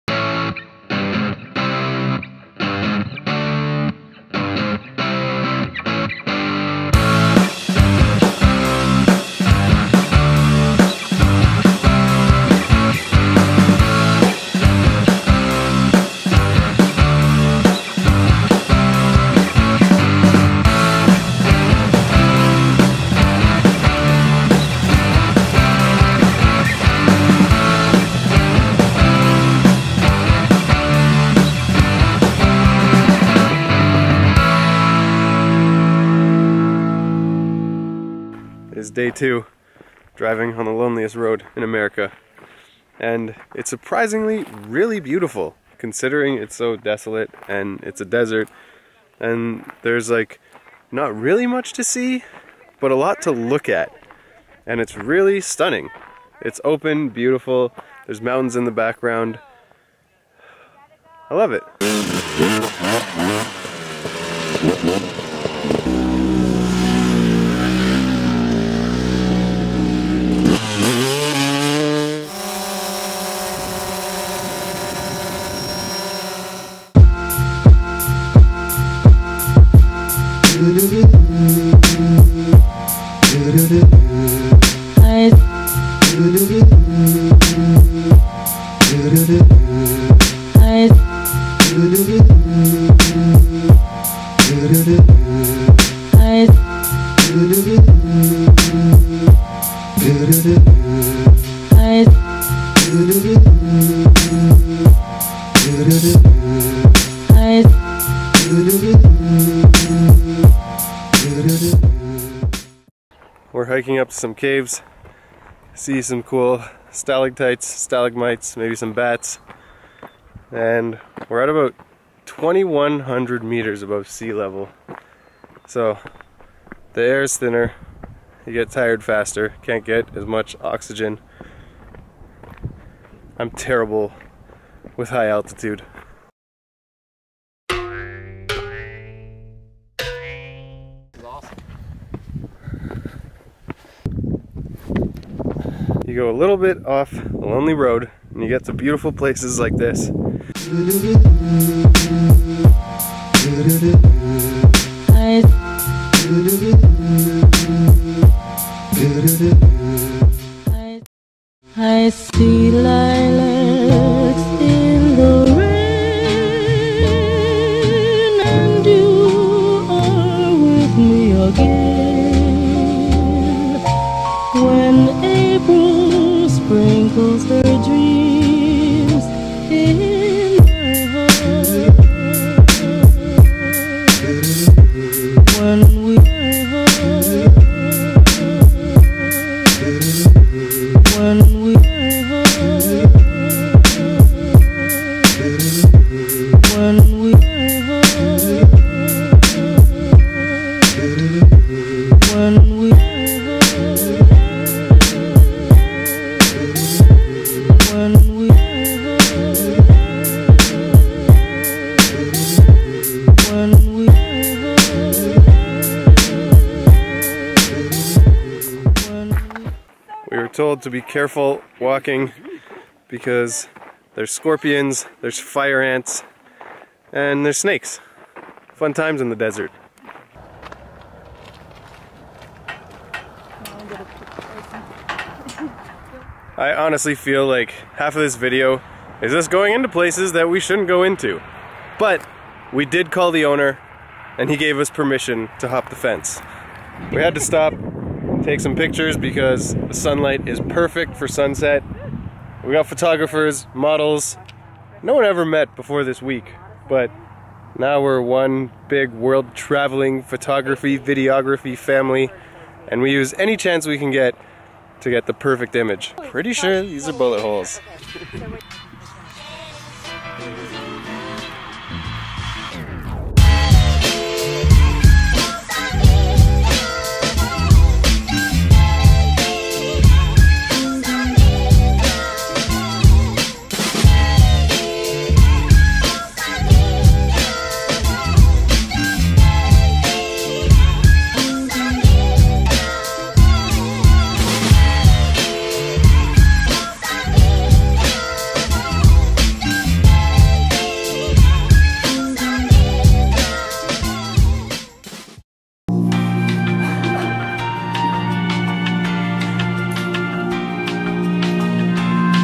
Nevada Road Trip in 4K - Part 2
This is PART TWO of TWO videos where Siya toured in a van with a bunch of fun travellers and drove along the loneliest highway in America. Highway 50 in Nevada It is literally named "The Loneliest Highway" but was far from lonely with this group of rebels. The road is surrounded by incredible sites as you will see with this bangarang drone footage in 4K!

A special thank you to Travel Nevada for putting this itinerary together. It was a blast! Follow on Twitter and Instagram: @TravelNevada

WHERE YOU CAN FIND SIYA